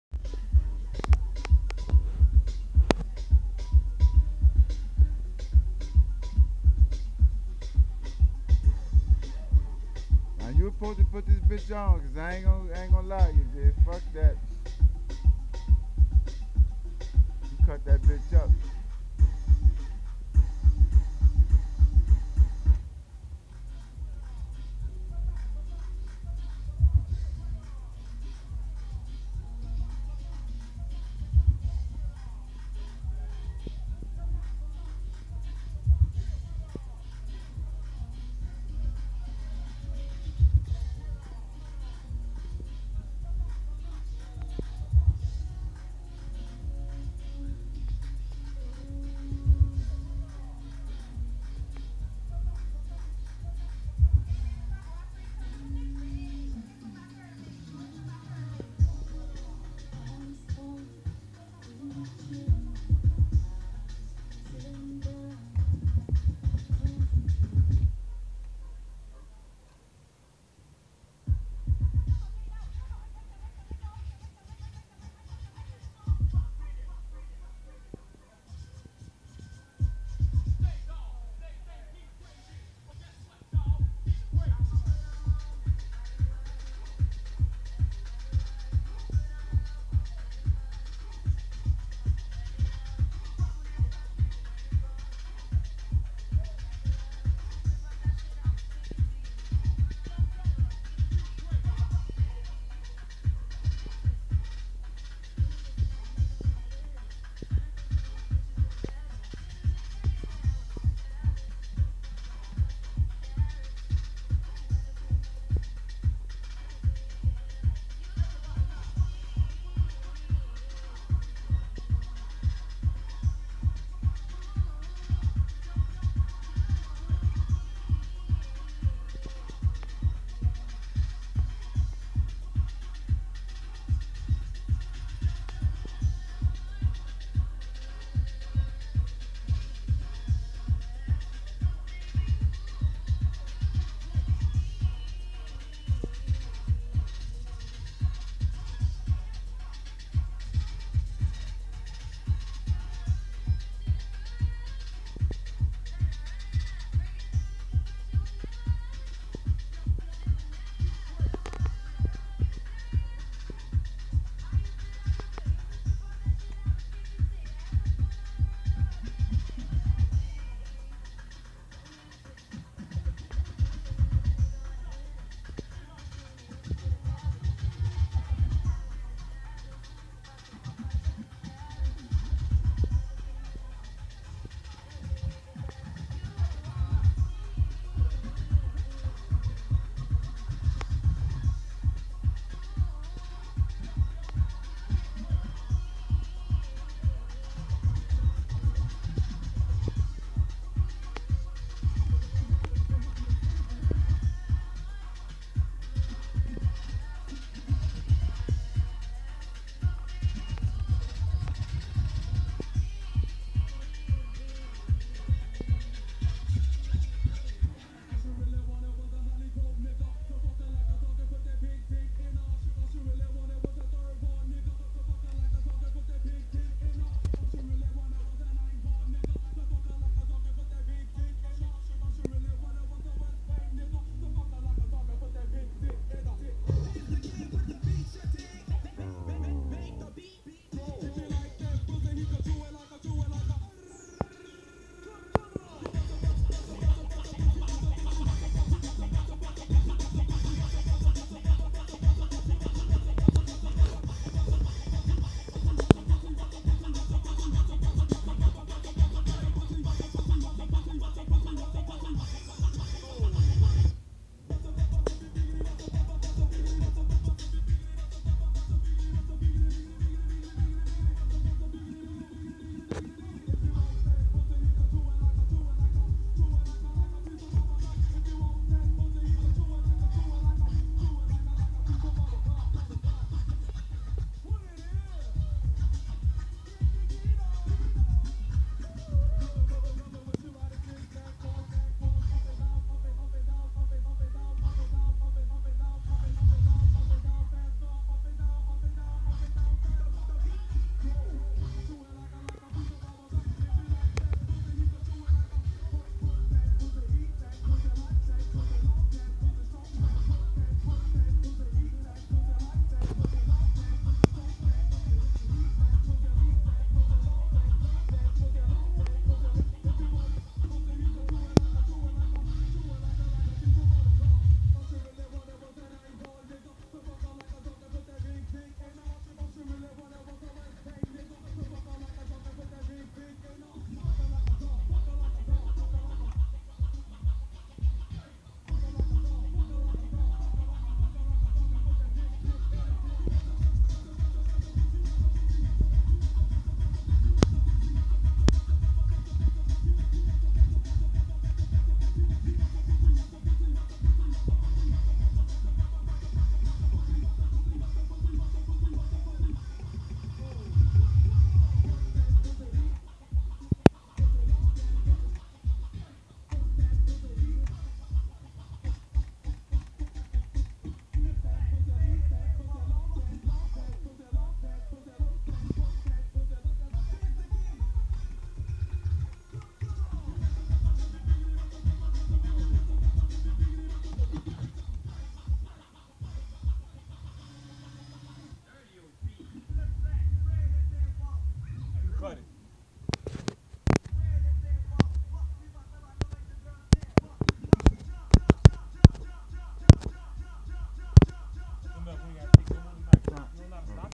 GoDj Scratchemann pres Mixin,cuts,scratches.with N.O.Bounce
Thanking Big Chuck,for hiring me to represent Midcity for a party @The Playhouse on Ulloa and Tulane.displaying next level skills,cuts,scratches and the new thing the booty rockers love..."the beat punched live"